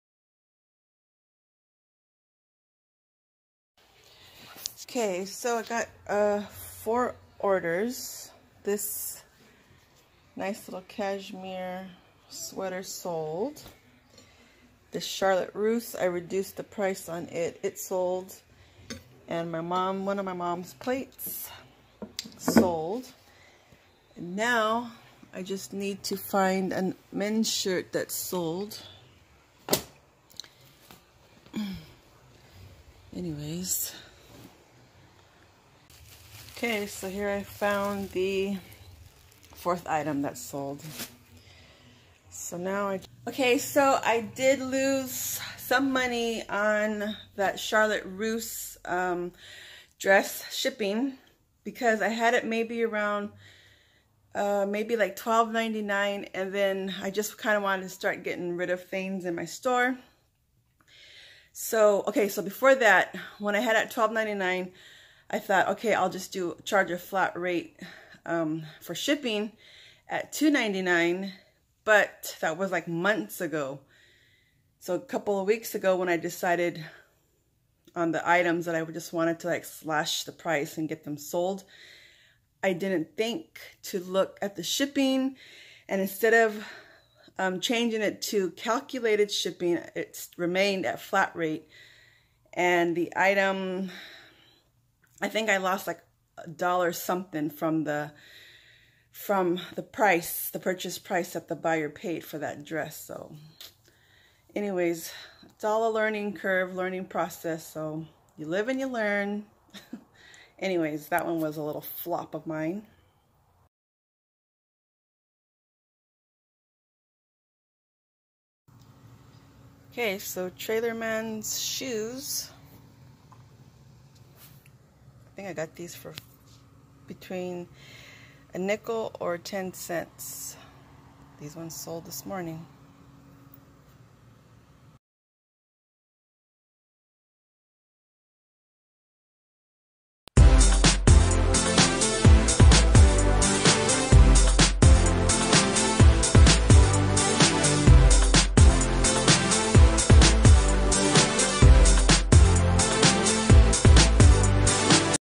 What sold on Ebay | Ebay Starter Store
Here is what sold this far on my Ebay Starter Store.